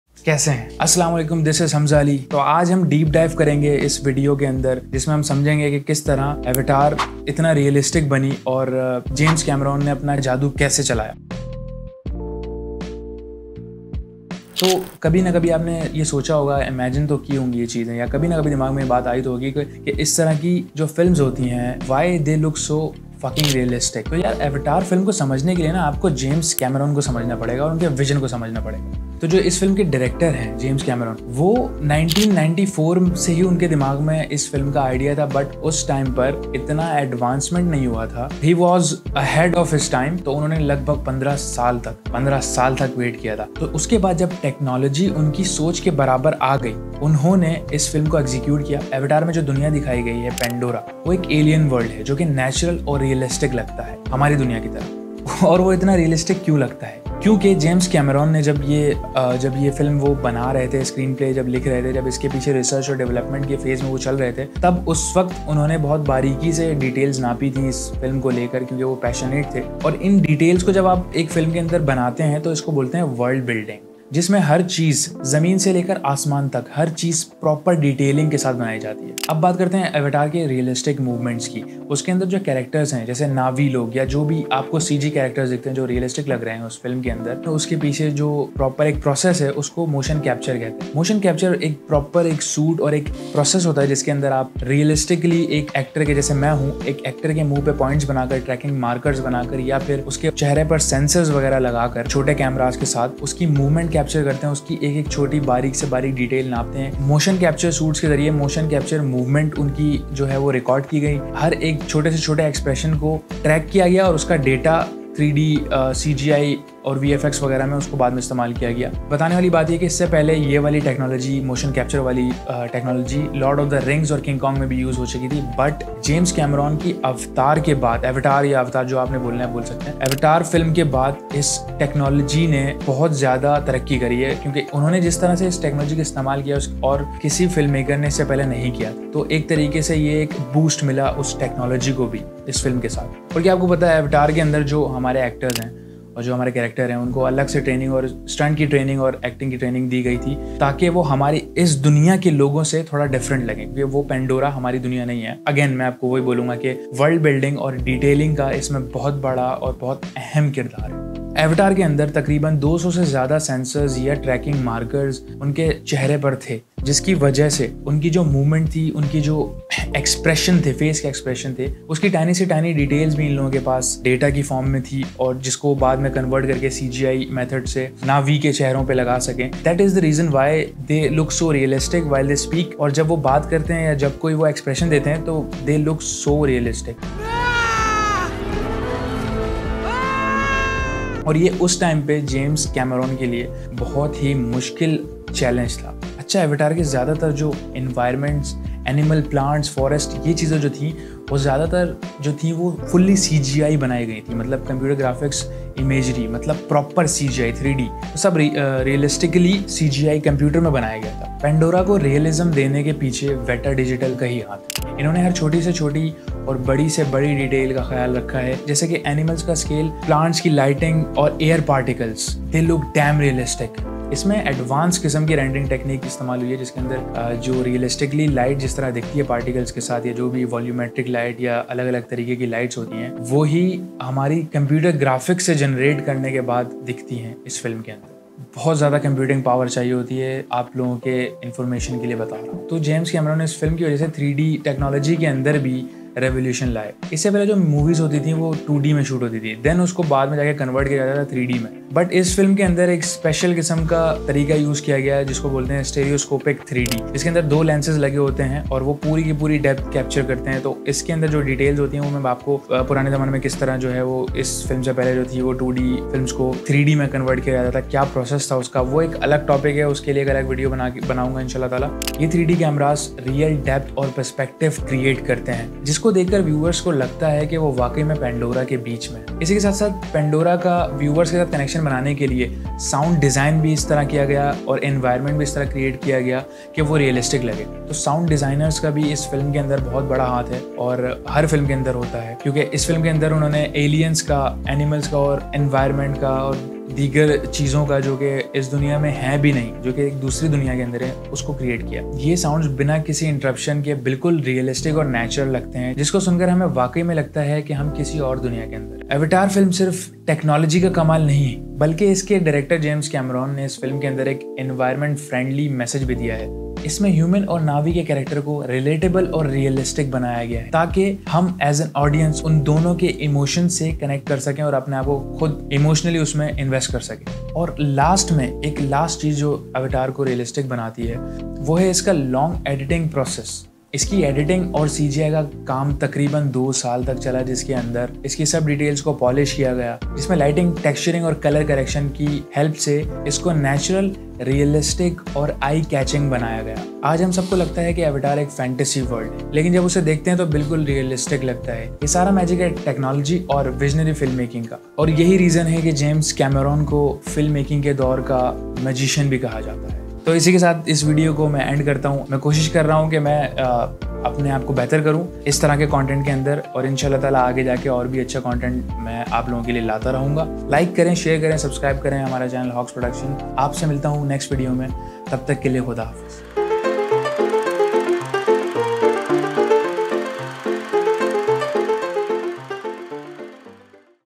Avatar - Technical Breakdown | Hamza Ali | Hawks Production
In this episode, Hamza from Hawks Production delves into the technical brilliance of James Cameron’s Avatar. Join him as he breaks down the revolutionary VFX, motion capture, and CGI that brought the world of Pandora to life, setting new standards in cinematic technology.